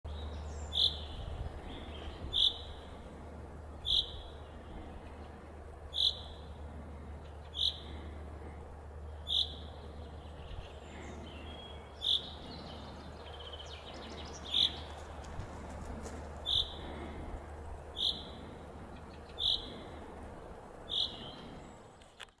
Зяблик - засыпает, но чирикает ;)
Зяблик - засыпает, но чирикает ;) Finch bird - falls asleep, but chirps ;)